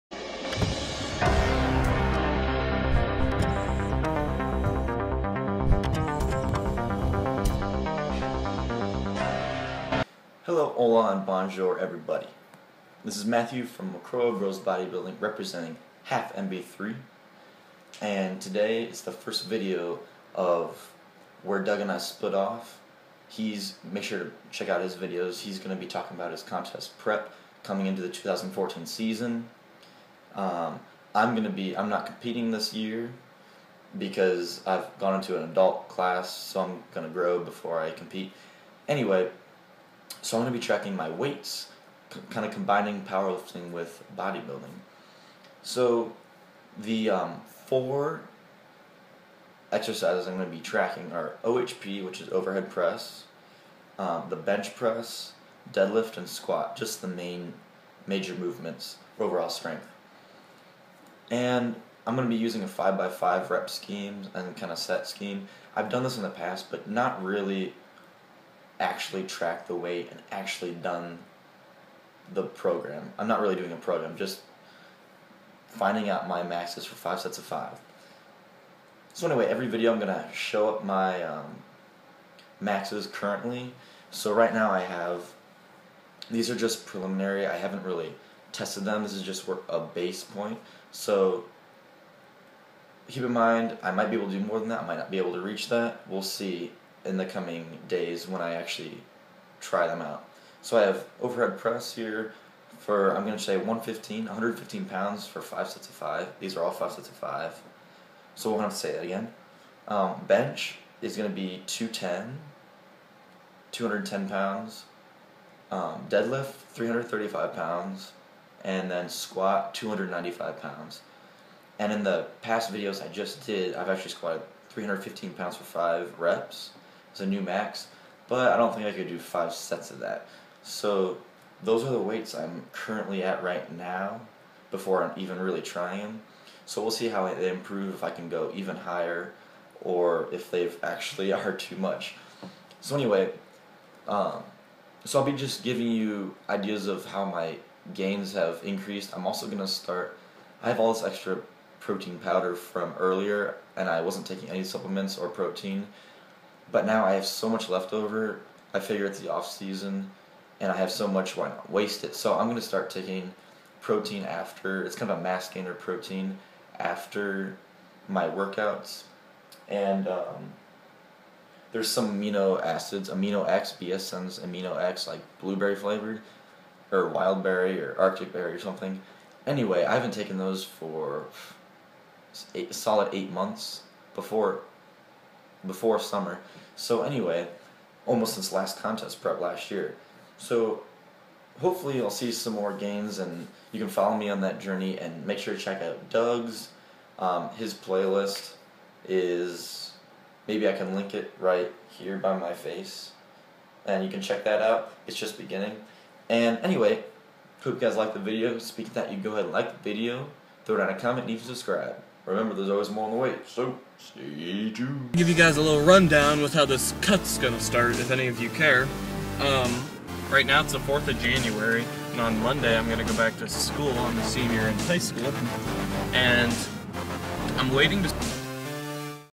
Intro to Matthew's Powerlifting and Bodybuilding Offseason
I explain what the next few months are going to be all about.  I show my main lift weights currently and what I plan to do to increase them.